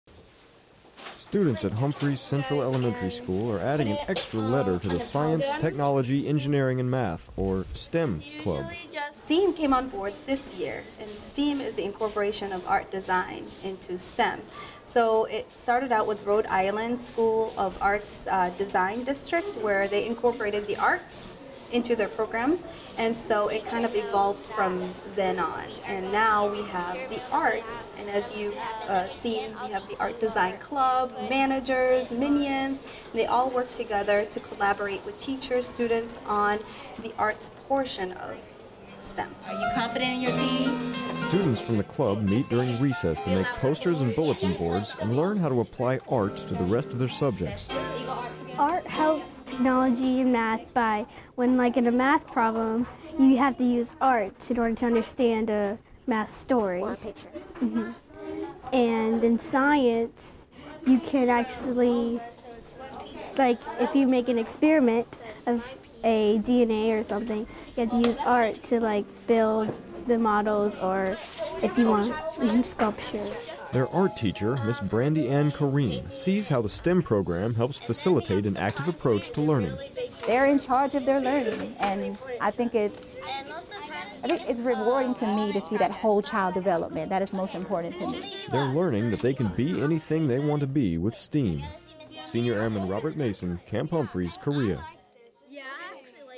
AFN Osan - STEAM
Creativity allows students to think beyond the limits of a curriculum.  Senior Airman Robert Mason is at Camp Humphreys to explain more about a new program.